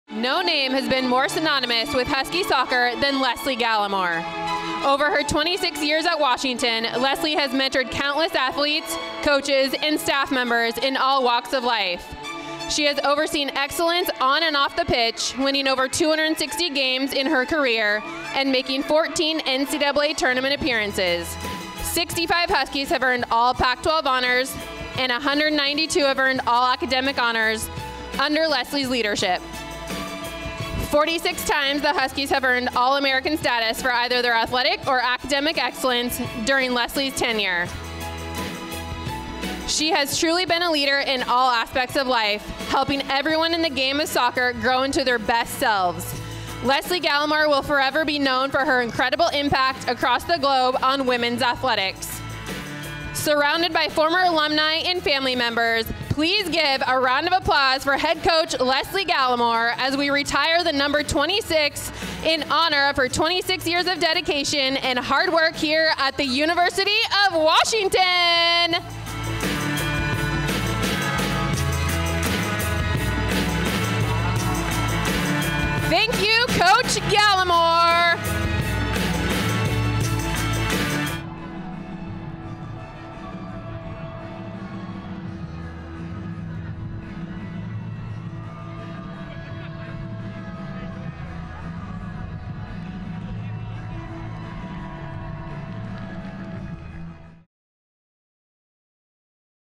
WSOC: UW retires No. 26 jersey for longtime head coach Lesle Gallimore
Before Friday's Washington State-Washington women's soccer match in Seattle, the Huskies retired the No. 26 jersey for head coach Lesle Gallimore, who is set to retire following a remarkable 26-year career. Gallimore has led the Huskies to unprecedented success, including 14 NCAA Tournament berths and a pair of Elite Eight appearances.